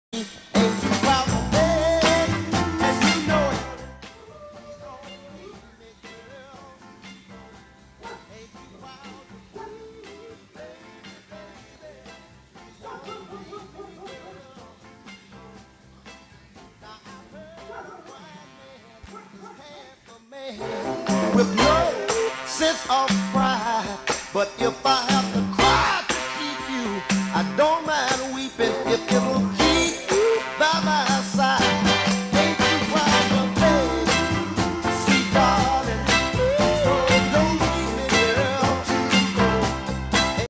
BandyBrat
Brandy Yelling at our neighbor with a bone in her mouth!!!